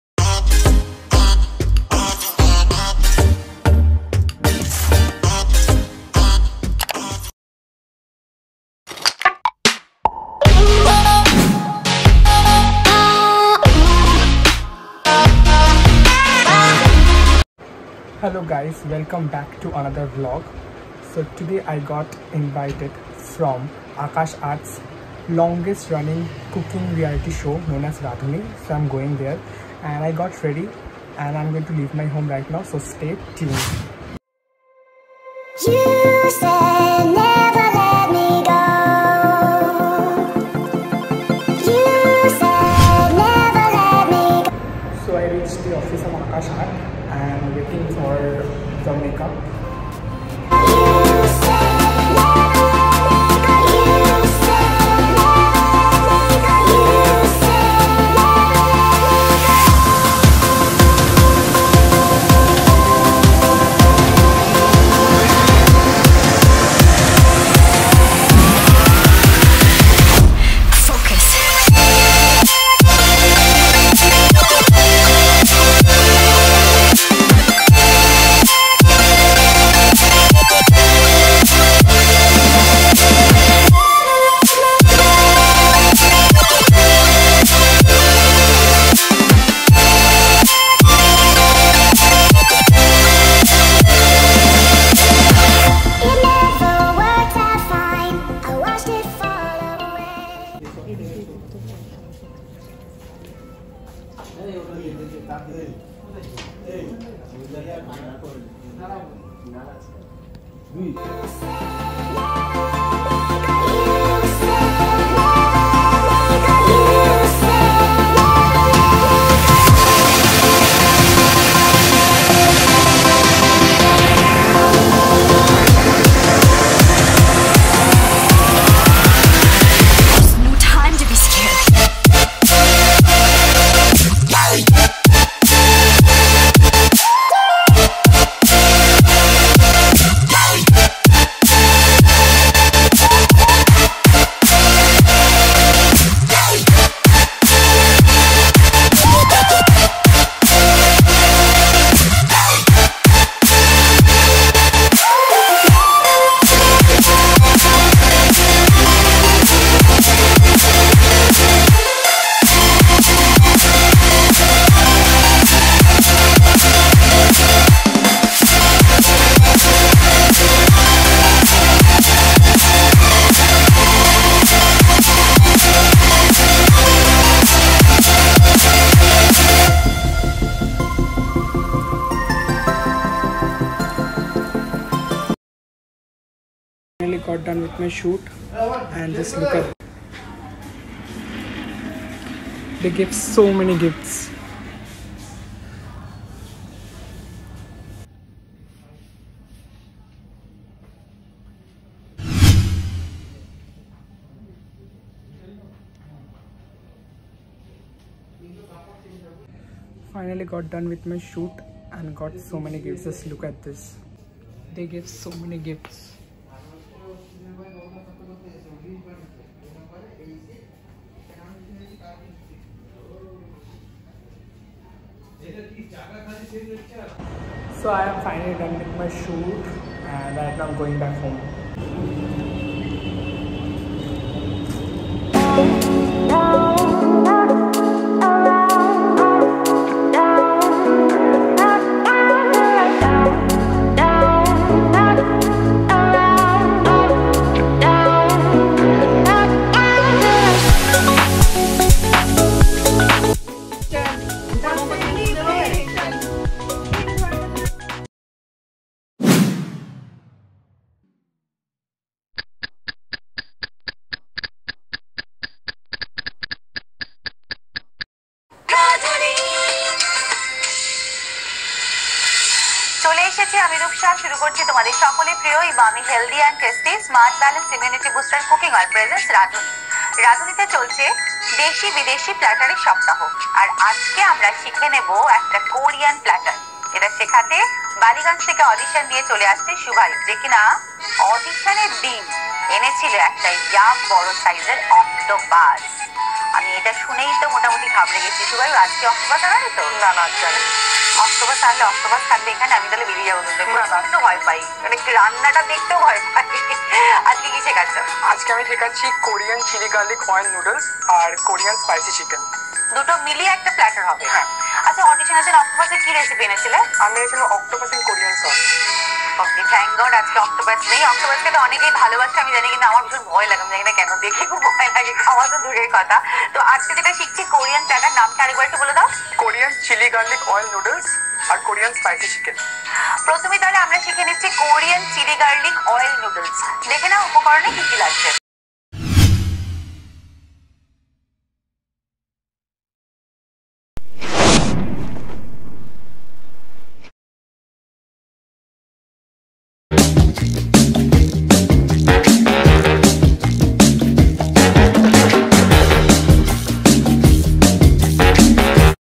Radhuni VLOG~My First Reality TV Show Debut~ Aakash Aath ~Radhuni
Hi Guys, I am back after a very long time.
And guess what I vlogged about my latest and first reality tv show - Radhuni.
For : Radhuni
Channel: Aakash Aath
Host : Rupsha Chakraborty
Director : Pompy Mukherji

Thank you everyone who have supported me all throughout my good and bad days, without your blessings and love this would not have been possible. And I am forever grateful to my family for their constant support.

Thank you Rupsha ma’am for being the amazing host and Pompy ma’am for being the amazing director and I would like to thank the entire team of Radhuni for all the support. 💜